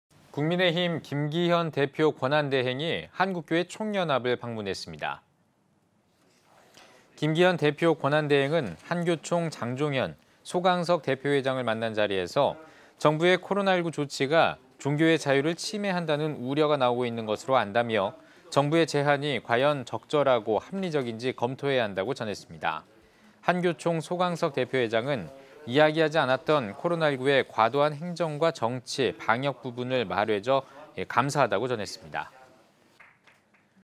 국민의힘 김기현 대표 권한대행, 한교총 내방 (이한승) l CTS뉴스
국민의힘 김기현 대표 권한대행이 한국교회총연합을 방문했습니다,

김기현 대표 권한대행은 한교총 장종현, 소강석 대표회장을 만난 자리에서 ‘정부의 과도한 코로나19 조치가 종교의 자유를 침해한다는 우려가 나오고 있는 것으로 안다’며 ‘과도한 정부의 제한이 과연 적절하고 합리적인지 검토해야 한다’고 전했습니다.

한교총 소강석 대표회장은 ‘이야기하지 않았던 코로나19의 과도한 행정과 정치 방역 부분을 말해줘 감사하다’고 답했습니다.